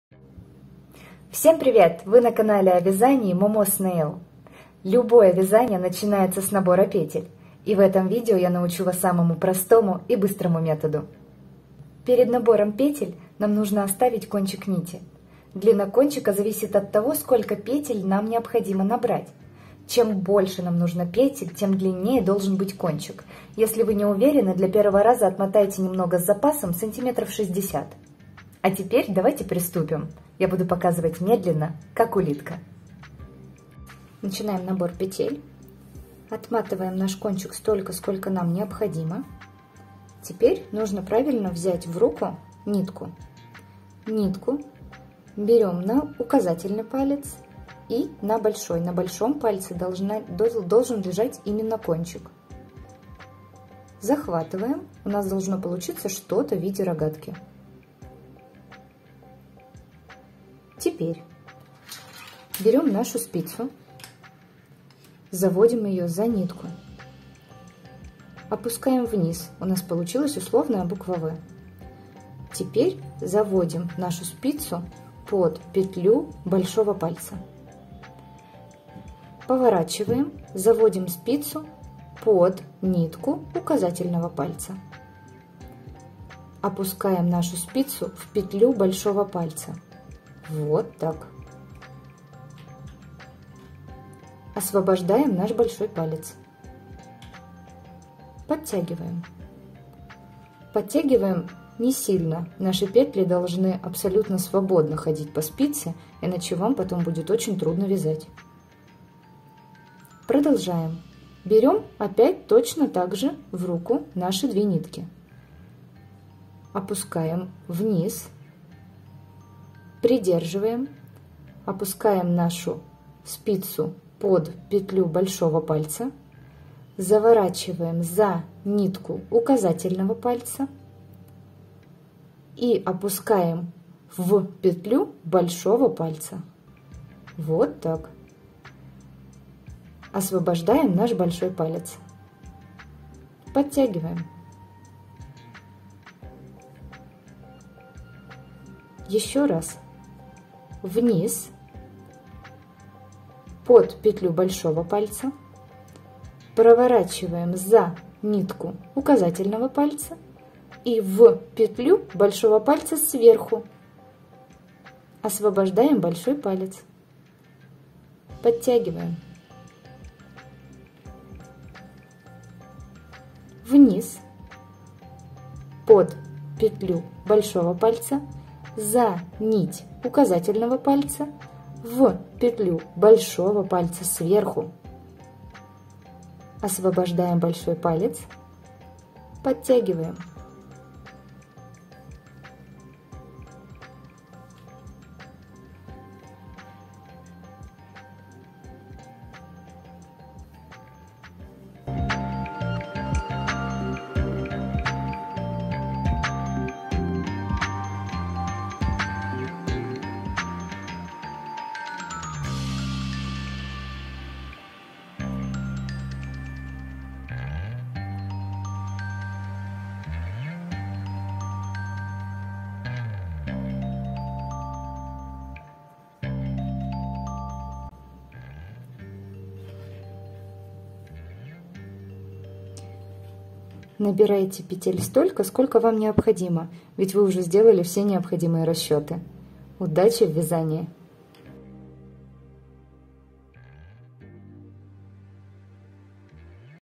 Как быстро набрать петли на спицы | МК по вязанию для новичков
Классический и самый простой набор петель на спицы. Показываю медленно и понятно для новичков.
У вас точно получится!

Возможно вы искали